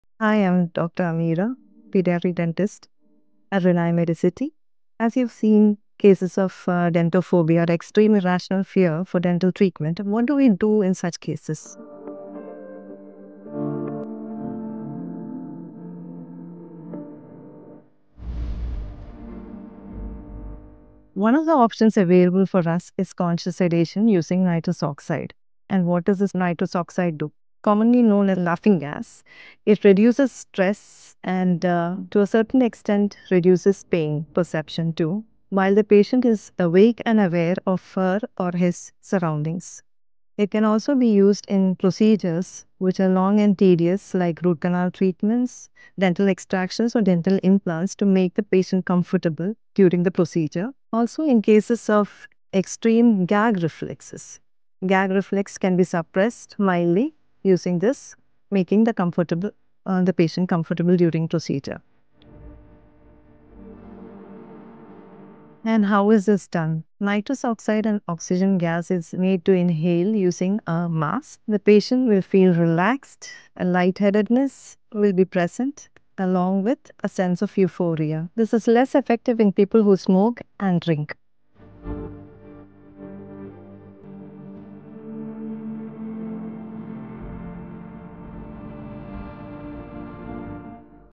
കുട്ടികളിലെ ദന്തഭീതി മാറ്റാം | Dr. Ameera Explains Conscious Sedation
ദന്തചികിത്സയോട് ഉള്ള ഭയം — പ്രത്യേകിച്ച് കുട്ടികളിൽ — യാഥാർത്ഥ്യമാണ്.

എന്നാൽ, ആധുനിക ചികിത്സാ രീതികൾ ഉപയോഗിച്ച് എങ്ങനെ Dentophobia (ദന്തഭീതിയെ) നേരിടുന്നുവെന്ന് പെഡോഡോണ്ടിക്സ് & പ്രിവന്റീവ് ഡെന്റിസ്ട്രി വിഭാഗത്തിലെ സീനിയർ ഡെന്റൽ സർജൻ ഡോ. അമീറ അഹ്മദ് ഇസ്മായിൽ വിശദീകരിക്കുന്നു.

രോഗികളുടെ സ്ട്രെസ് കുറക്കുകയും വേദന ശമിപ്പിക്കുകയും ചികിത്സയെ കൂടുതൽ ആശ്വാസകരവും ഭയമില്ലാത്തതുമാക്കുകയും ചെയ്യുന്ന കോൺഷ്യസ് സഡേഷൻ എങ്ങനെ ഇതിന് മികച്ചൊരു പരിഹാരമാകുന്നു എന്നും ഡോ. അമീറ വ്യക്തമാക്കുന്നു.